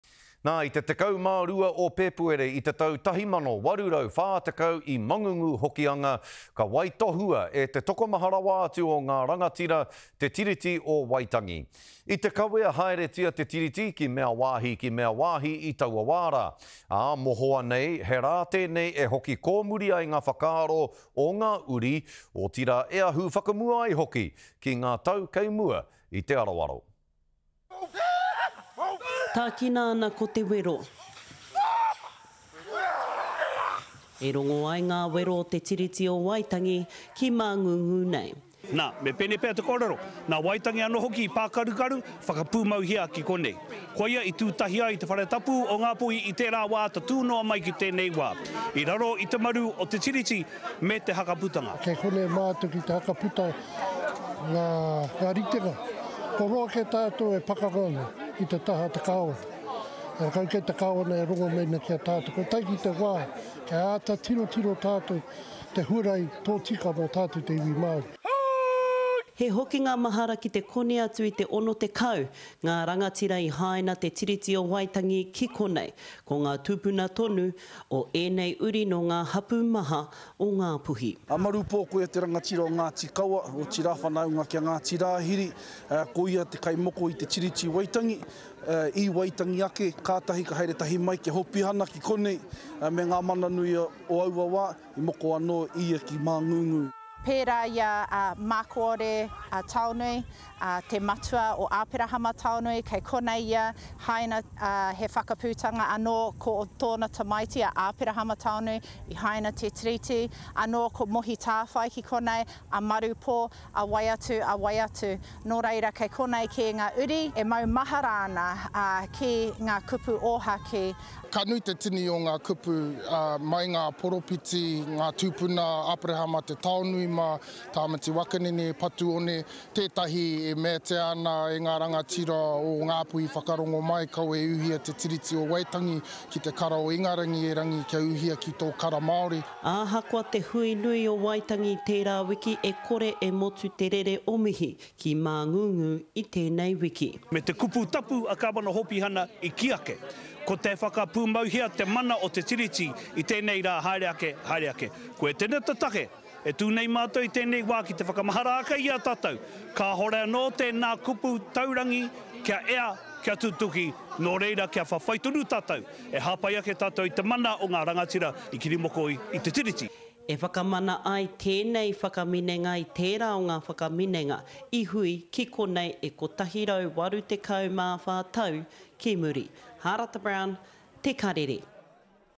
Te Tiriti o Waitangi commemorations continue in Te Tai Tokerau
It's at Māngungu, Hokianga on the 12th of February when the highest number of rangatira signed Te Tiriti o Waitangi, as it was taken to various parts of the country. The annual commemoration in the North continues to be a time to reflect and learn while moving forward.